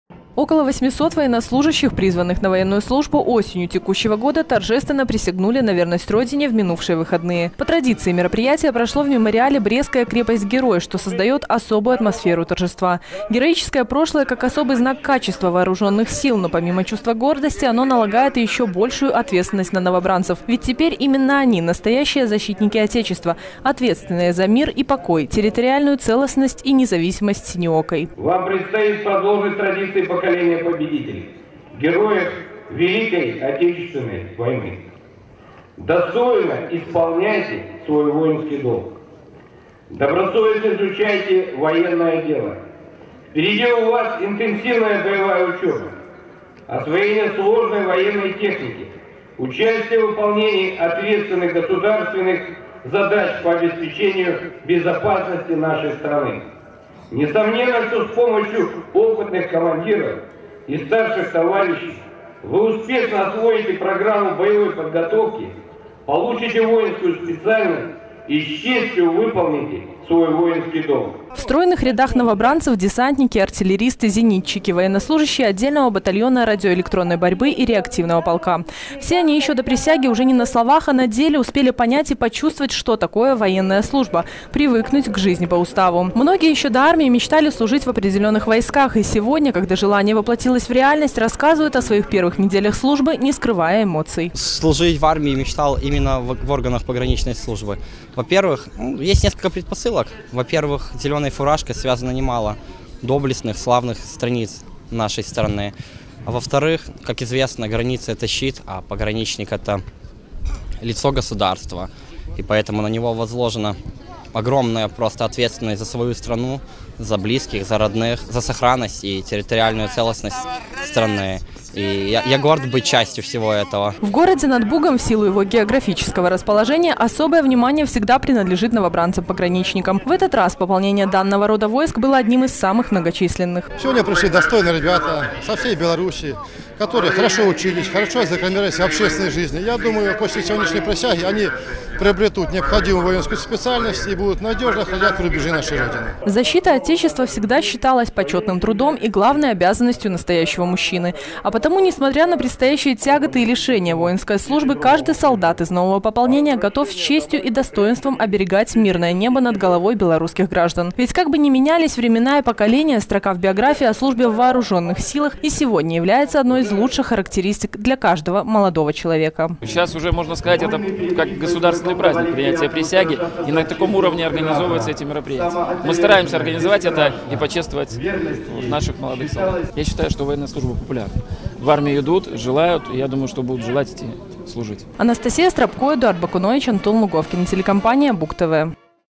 2014-12-15 г. Брест. Областная присяга. Телекомпания Буг-ТВ.
13 декабря в соединениях и воинских частях Вооружённых Сил Республики Беларусь прошли торжественные мероприятия, посвящённые принятию Военной присяги новым пополнением. По всей стране 9 тысяч солдат произнесли слова торжественной клятвы и присягнули на верность Родине.
В торжествах приняли участие представители министерства обороны, местных органов власти, общественных организаций, ветераны, родные и близкие военнослужащих, призванных в ряды белорусской армии для прохождения срочной военной службы и службы в резерве. Берестейские новобранцы один из главных дней своей военной жизни провели на священной земле Брестской крепости.